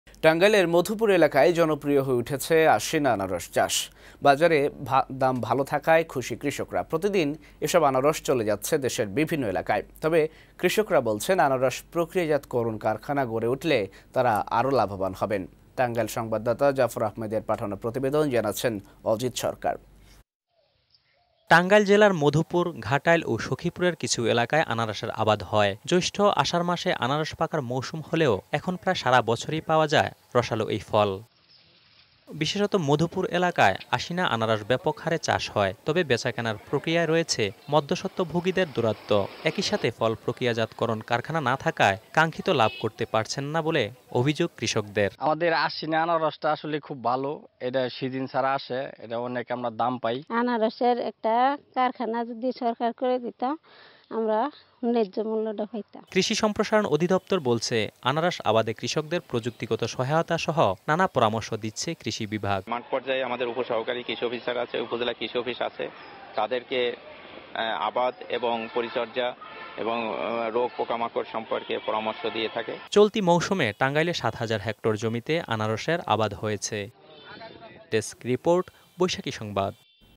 মধুপুরে জনপ্রিয় হয়ে উঠছে আশ্বিনা আনারসের চাষ | Tangail Pine Apple Cultivate
মধুপুরে জনপ্রিয় হয়ে উঠছে আশ্বিনা আনারসের চাষ | Tangail Pine Apple Cultivate | Ajit | 03Nov19